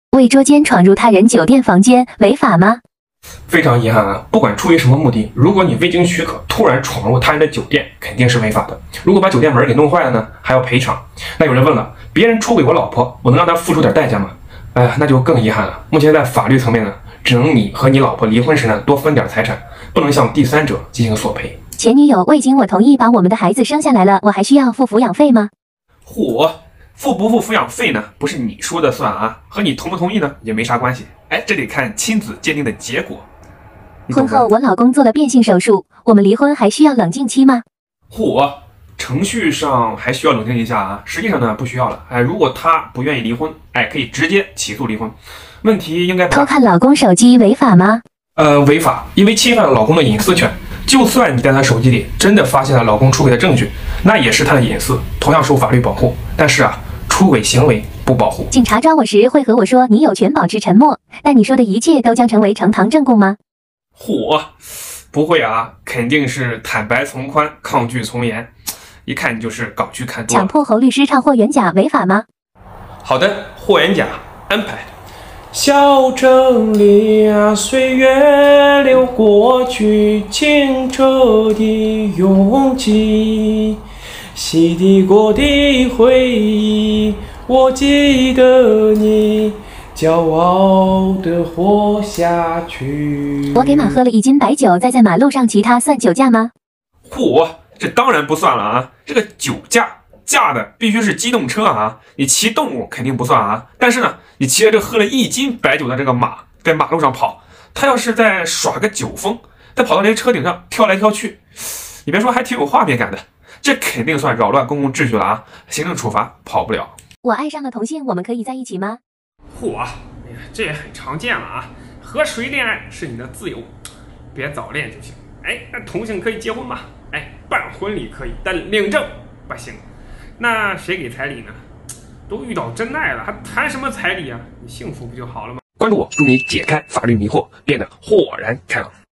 捉奸进酒店违法吗？【沪上魔都最最最正经律师】
魔都最最最正经律师，上海财经大学法学硕士，搞笑方式硬核普法
业务方向：投融资、并购、基金、上市、企业法律顾问以及重大民商事业务